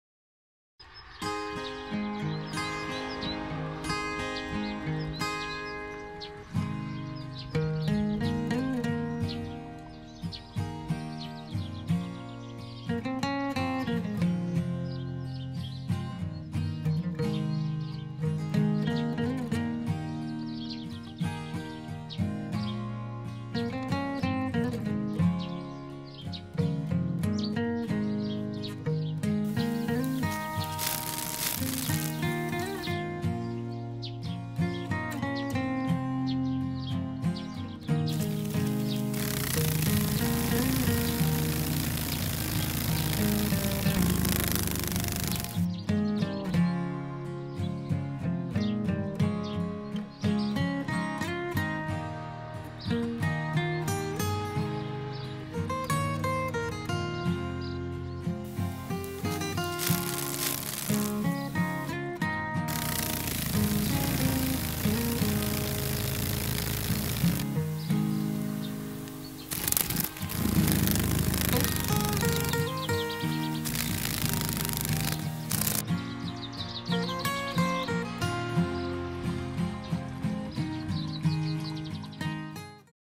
Dragons & Damsels
A very short video made in the garden where a dragonfly and some damselflies were flying around.  The general shots of the pond were shot using an iPhone 12 Pro MAX with the Filmic Pro application at 10bit V-log, whilst the shots of the dragons and damsels was shot at 60fps on my Nikon Z7ii with a 300mm lens and then slowed down in post processing.